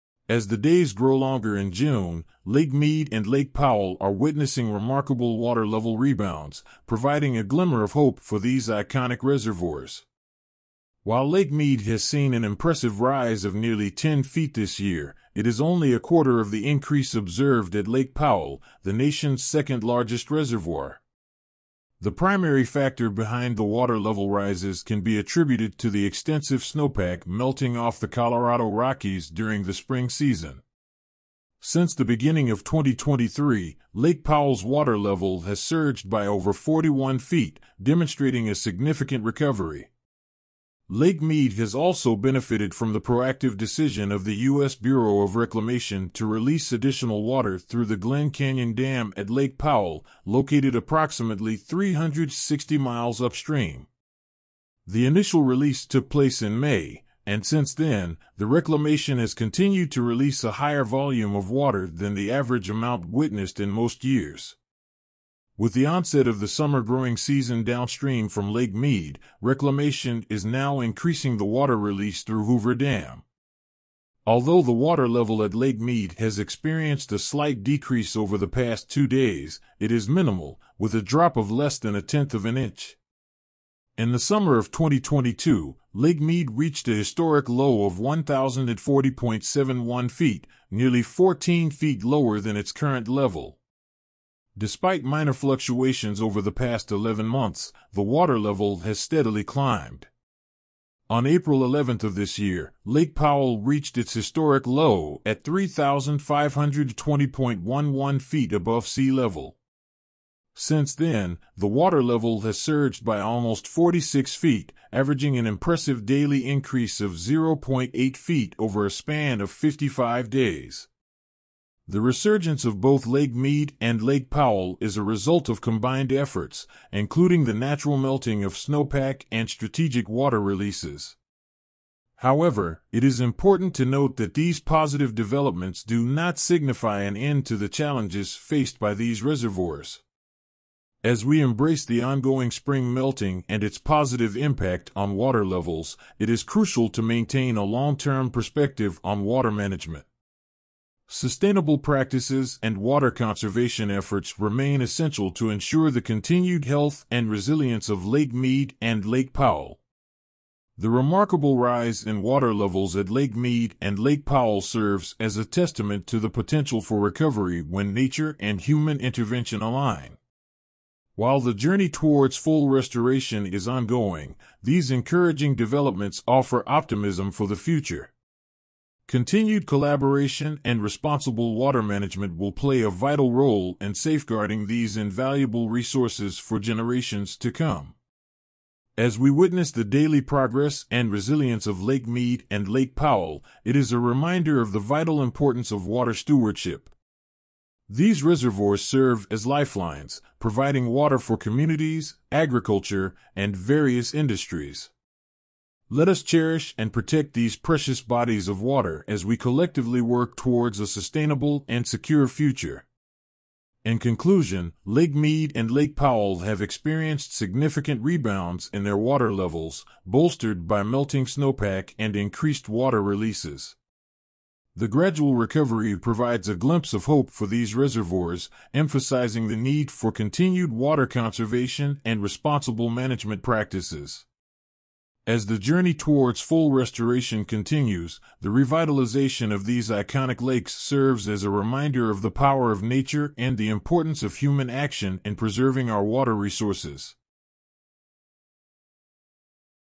Lake Powell continues meteoric rise
Lake Mead water level rise stalls: Lake Powell continues meteoric rise